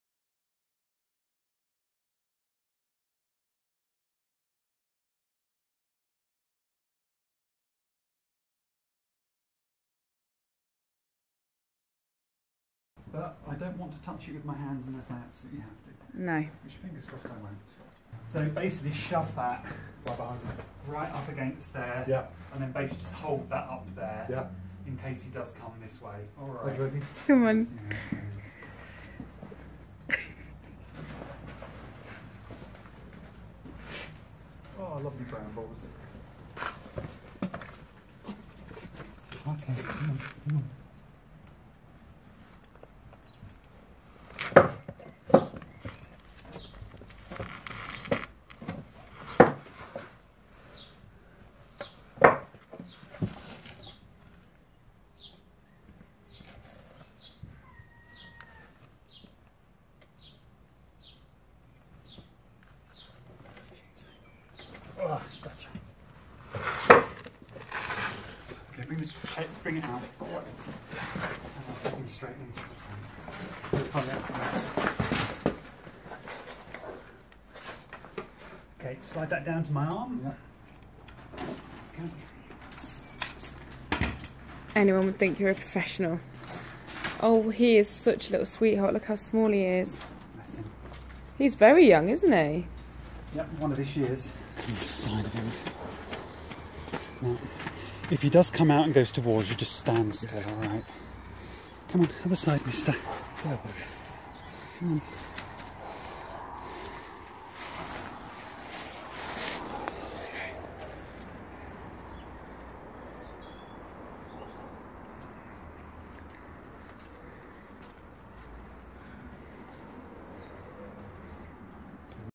Badger Rescued From 30ft Gap
A cute badger which fell from an embankment was rescued by volunteers from a narrow gap in a shocked owner’s patio garden.

East Sussex Wildlife Rescue & Ambulance Service caught and released a young badger which had fallen 20-30ft down an embankment onto a patio on Thursday.

Once on-site the badger was found curled up in a narrow gap between the house and the wall of the embankment.  Trevor had to squeeze down the gap with a dog grasper and blanket to secure the young Badger.

Trevor said: “It was difficult trying to get the dog grasper on the badger as he kept curling up tightly in ball, this is typical of badgers."